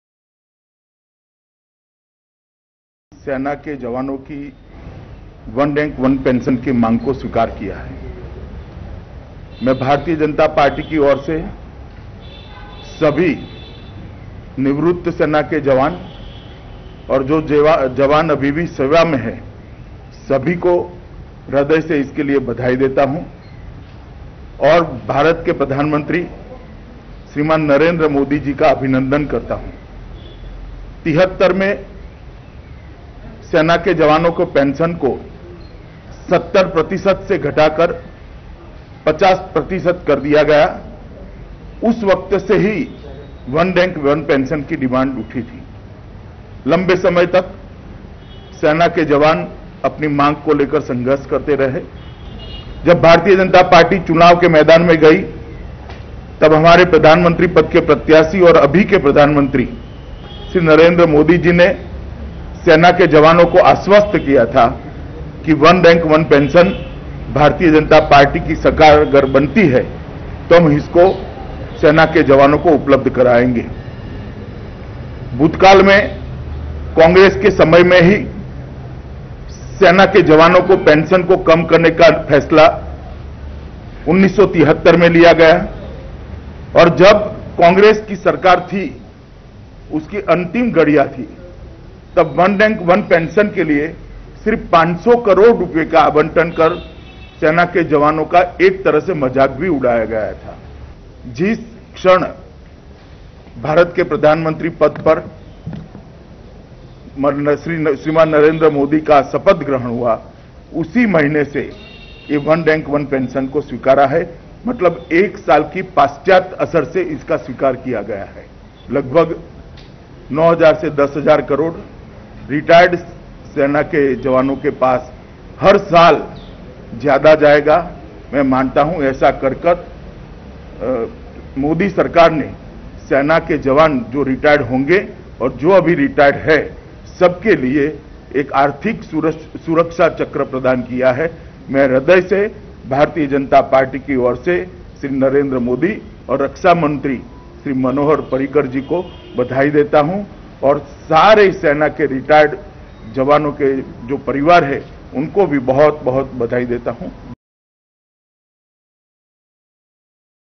Shri Amit Shah congratulates PM & Ex servicemen on implementation of OROP (05 Sep 2015)
Shri Amit Shah congratulating PM Narendra Modi on implementing  One Rank One Pension.(05 Sep 2015)